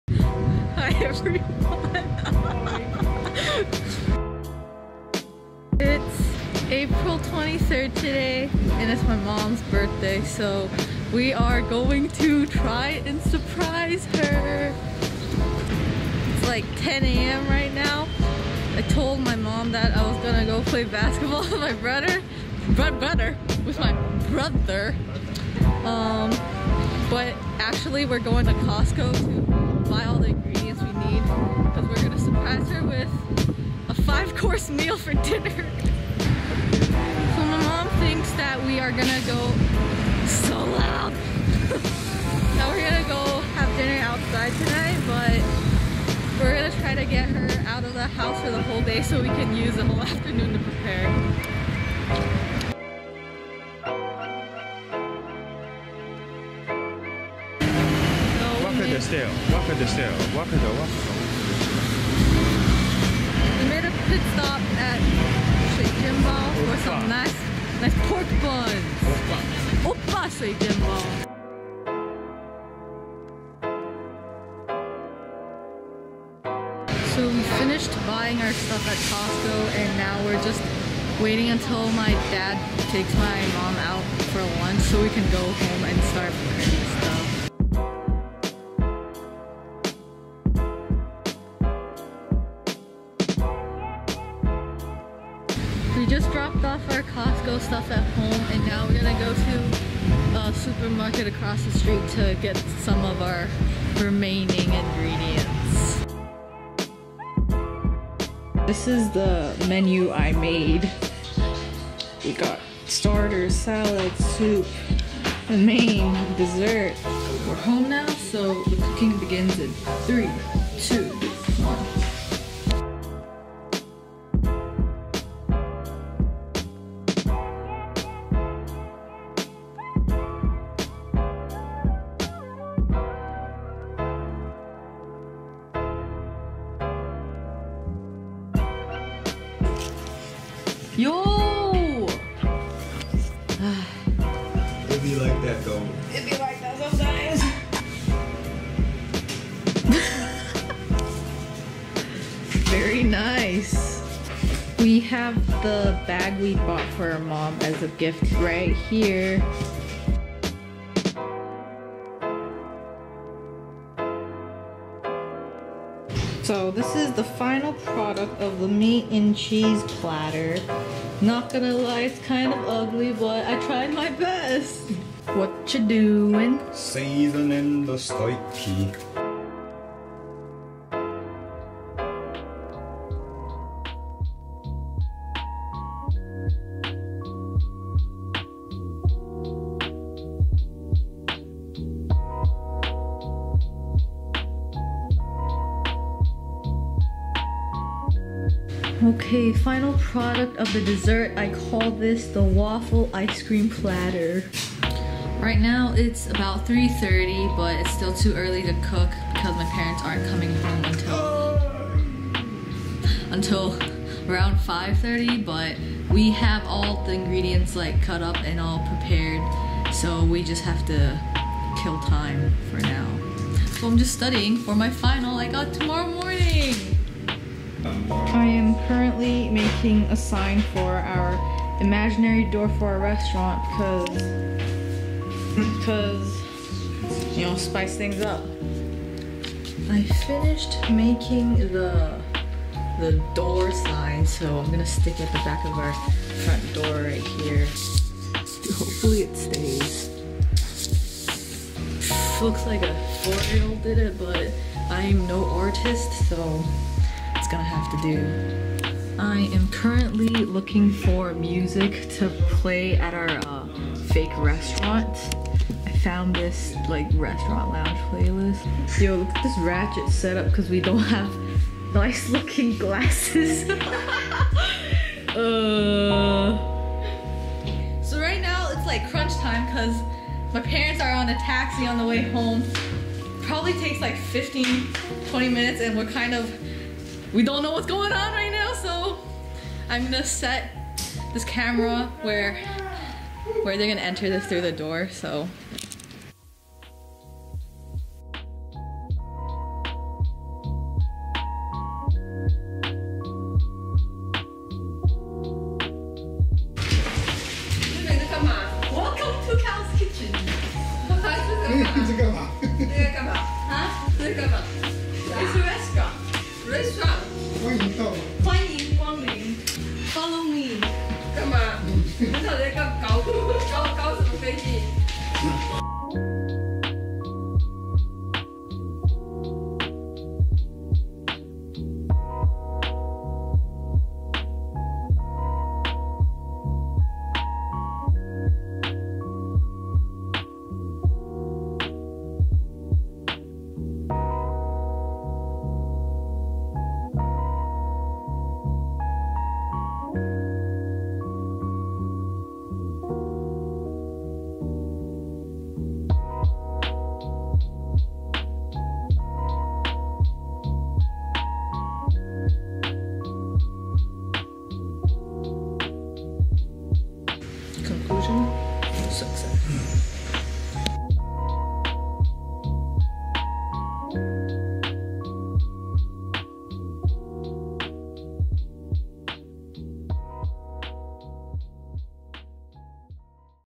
SURPRISED MY MOM FOR HER BIRTHDAY
My family and I decided to switch things up this year and surprise my mom for her birthday! Instead of having a nice meal at a restaurant like we always do, we made her dinner.